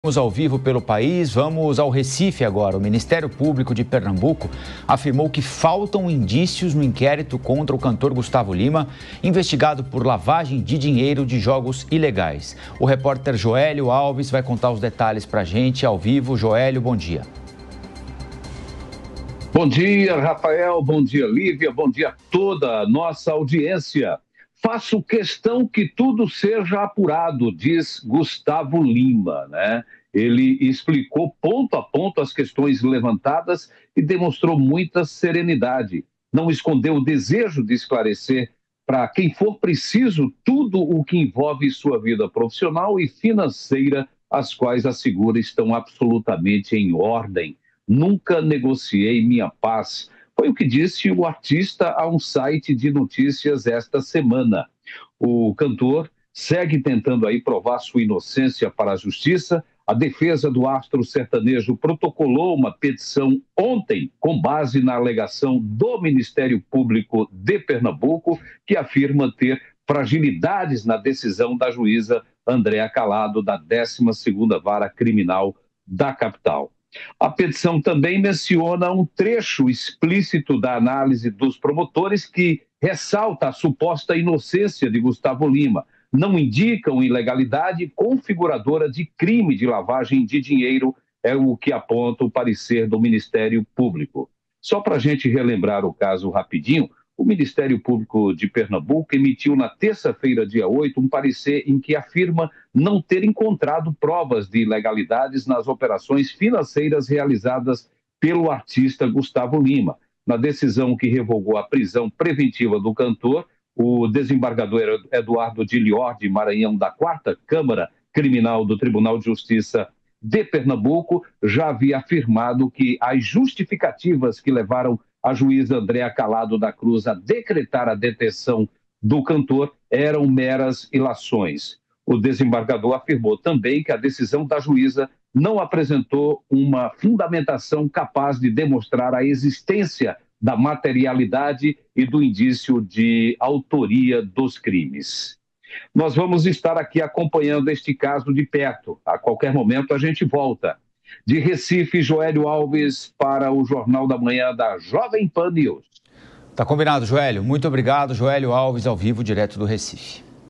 MPPE não vê crime em investigação contra Gusttavo Lima
O Ministério Público de Pernambuco (MPPE) disse que faltam indícios que demonstrem a prática de crimes no inquérito contra Gusttavo Lima, indiciado por lavagem de dinheiro proveniente de jogos ilegais no âmbito da Operação Integration.